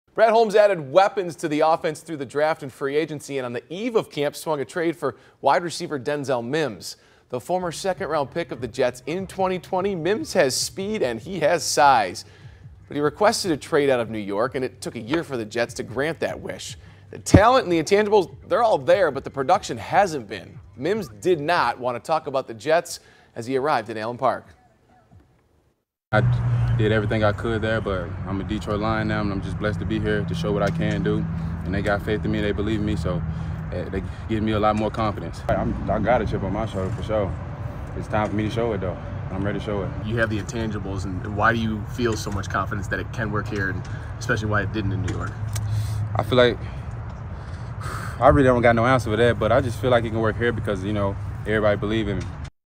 Denzel Mims done talking about Jets, expects success with Lions
Denzel Mims done talking about Jets, expects success with Lions because 'everybody believe in me' | Brad Galli has more